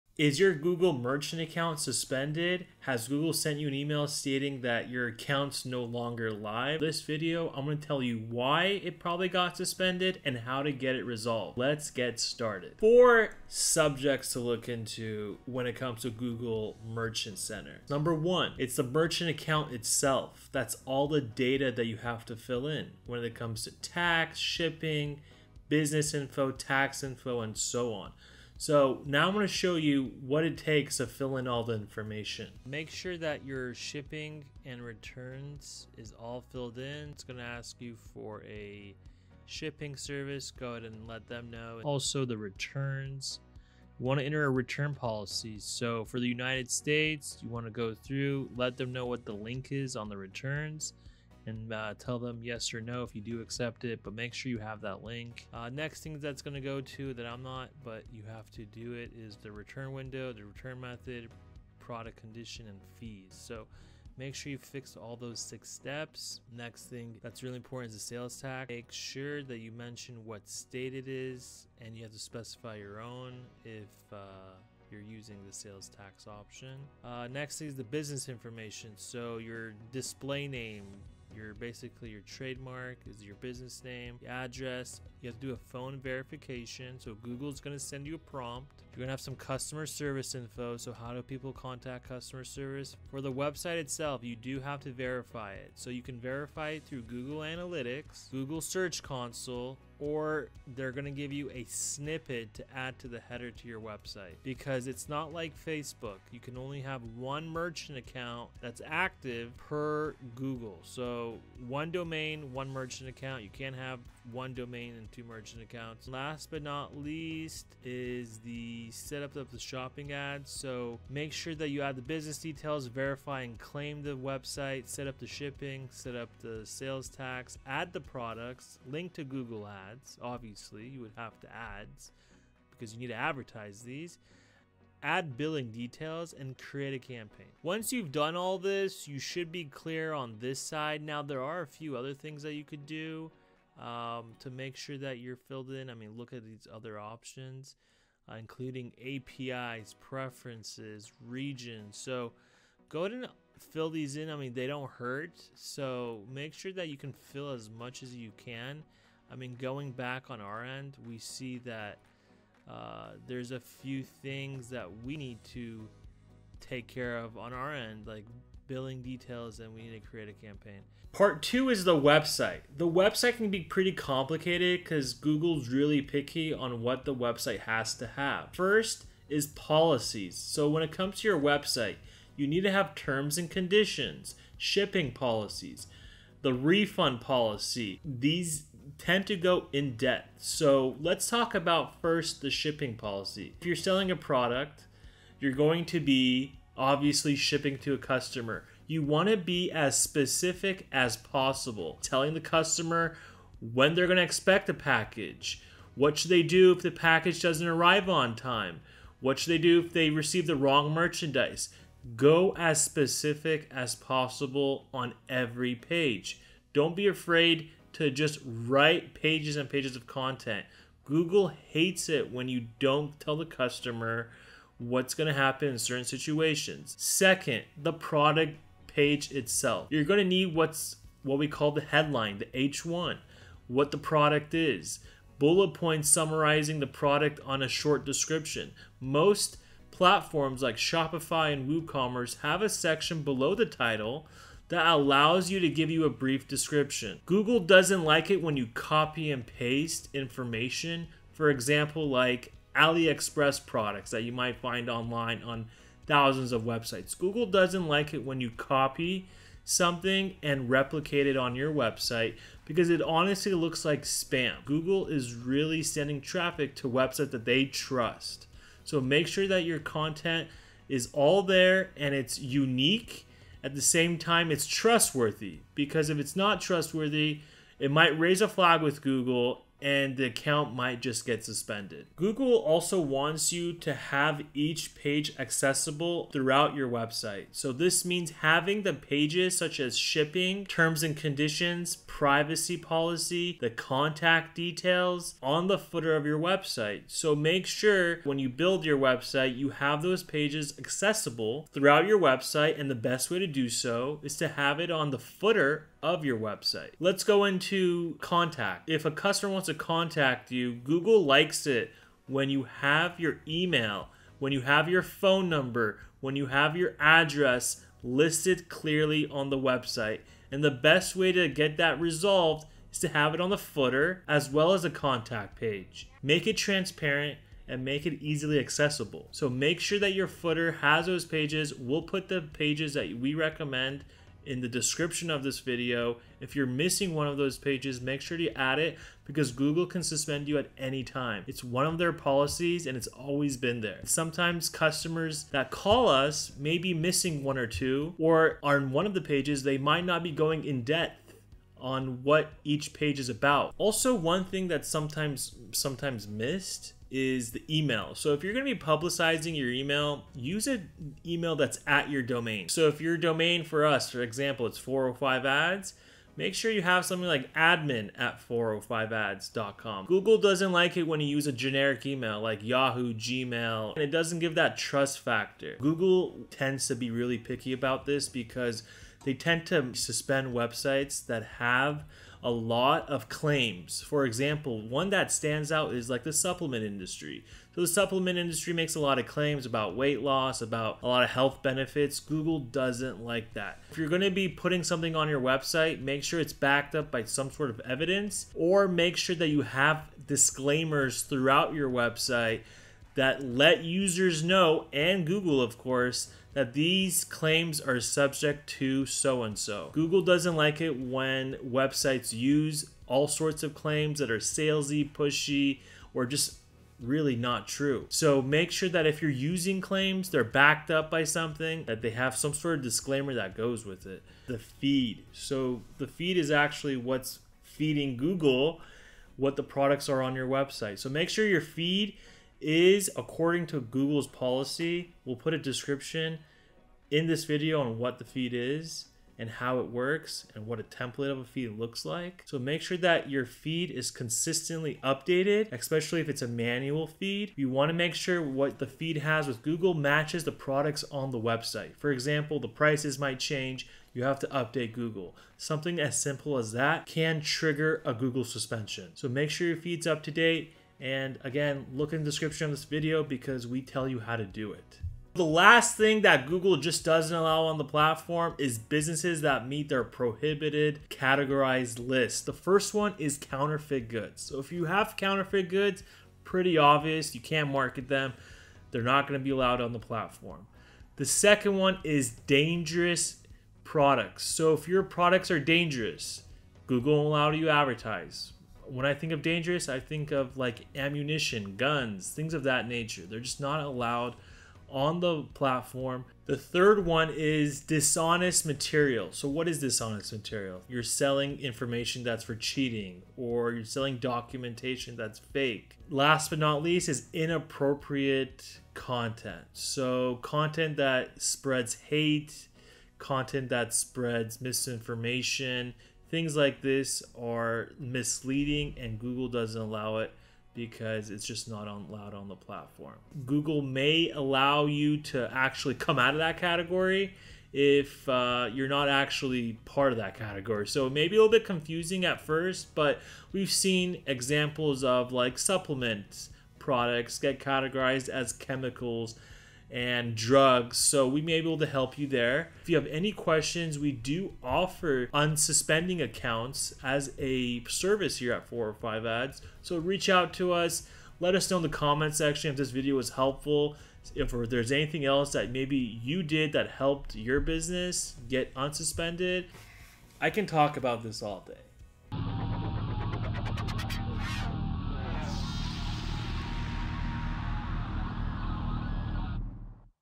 How to Fix a Suspended Google Merchant Center Account
📌📌🔥 Learn how to to unsuspend a Google Merchant Account 🔥
In this informative video, we dive deep into the process of unsuspending your Google Merchant account by leveraging the power of 405 ads. If you've ever encountered a suspension and struggled to reinstate your account, this step-by-step guide is exactly what you need!

⭐️Timestamps⭐️
0:18 All steps in Merchant Center need to be filled in
2:52 The website meeting Google's requirements
3:46 Product page requirements
4:43 Accessibility of your website's components
6:15 Disclaimers
7:04 The Google Merchant Center Feed
7:47 Prohibited Businesses

405 Ads link to the official Google Merchant Account suspension guidelines

📌 Key Takeaways 📌
In this article:
Product-level issues
Account-level issues
Request a review

Learn the exact reasons behind Google Merchant account suspensions.
Understand how 405 Ads can help reinstate your account effectively.
Follow a comprehensive step-by-step process to get your account back on track. Gain insights into monitoring ad performance and making necessary adjustments.
Submit your account for reconsideration with confidence.